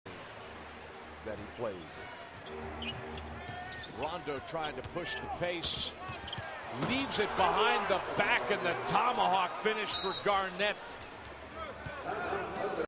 Rajon Rondo Behind The Back Pass To KG
Rajon Rondo with a sweet behind the back pass to Kevin Garnett.